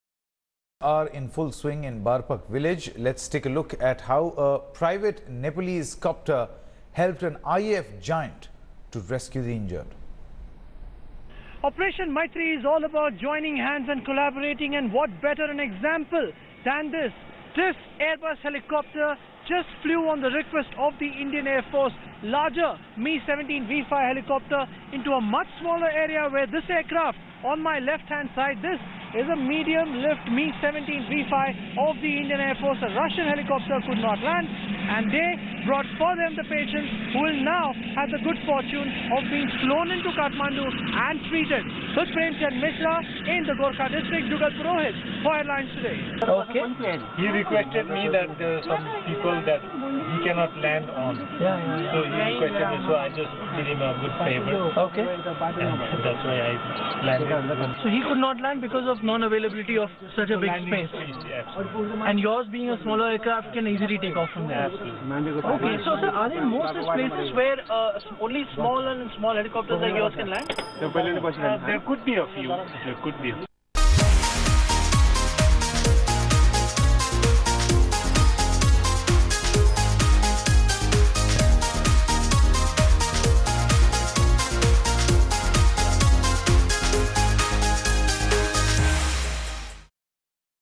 WATCH - How a small private chopper helped IAF giant Mi17 V5 chopper
This one is from the archives! During the Nepal earthquake, Indian armed forces responded to the neighbour's request and were deployed at short notice. While I was on ground, I saw how a unique chopper-to-chopper relationship evolved. While the IAF had bigger choppers, they could not go everywhere and that is where this small private helicopter helped.